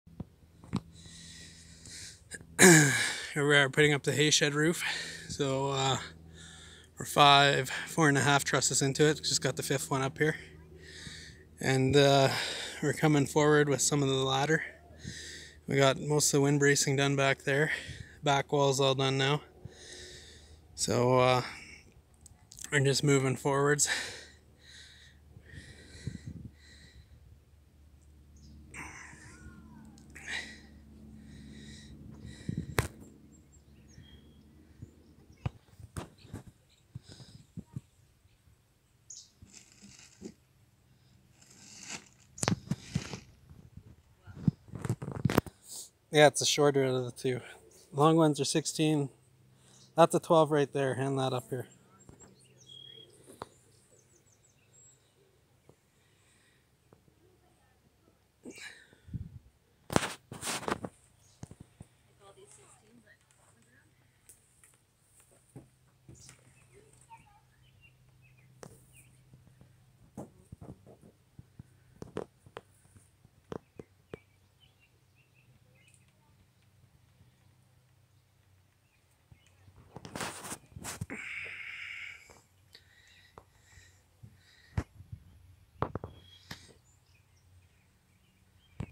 Mandy helps with the hayshed roof. Lifts the trusses with belarus t40a 420
5th truss up